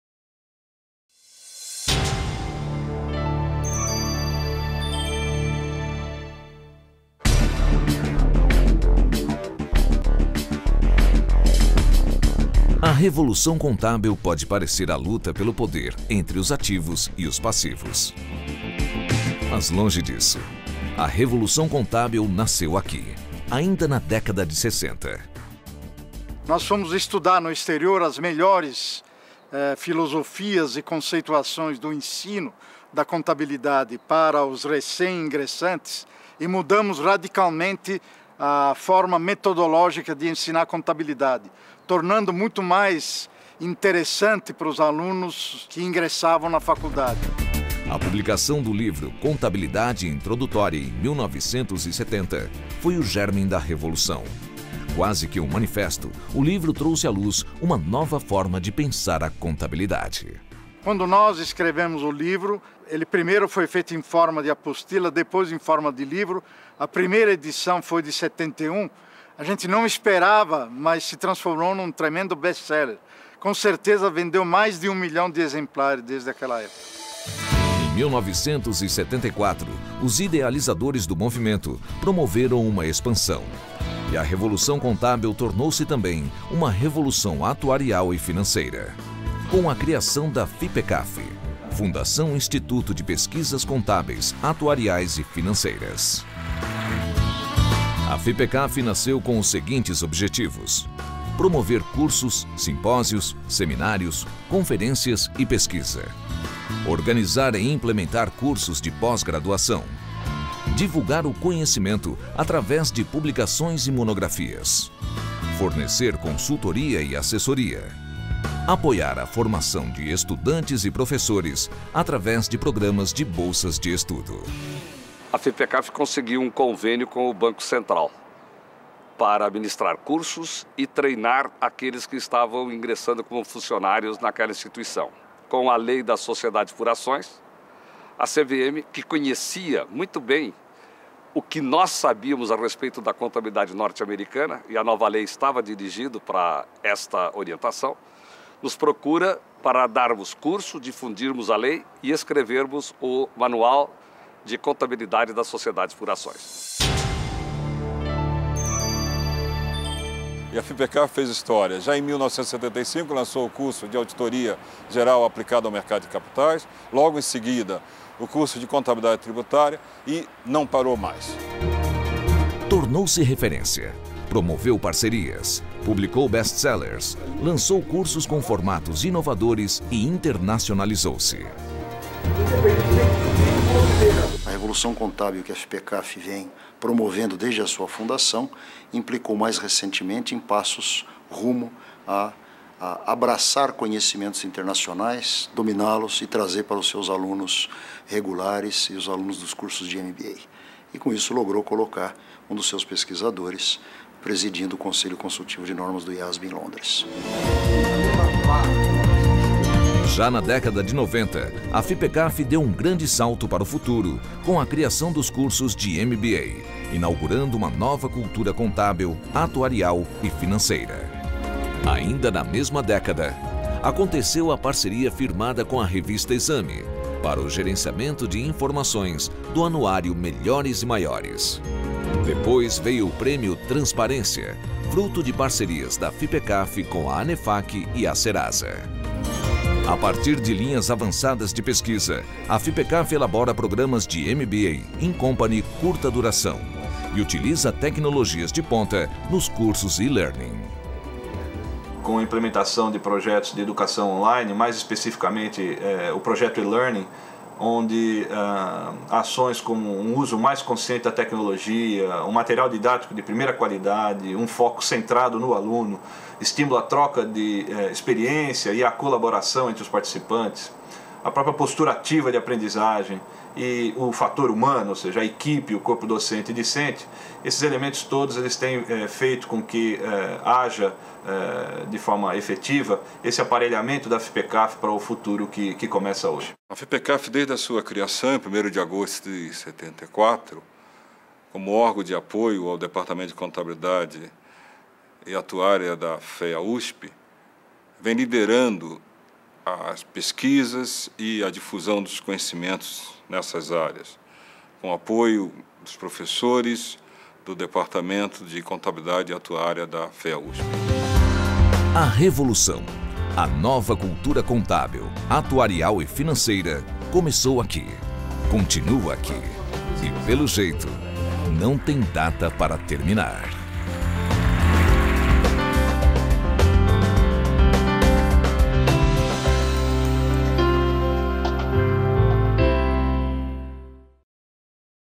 FIPECAFI - A Revolução Contábil
Em 1970, professores da Faculdade de Economia, Administração e Contabilidade da Universidade de São Paulo - FEA/USP colocaram no papel as suas experiências e lançaram o livro Contabilidade Introdutória. A publicação trouxe à luz um novo pensamento, quebrou tabus, varreu conceitos anacrônicos. Começava a Revolução Contábil.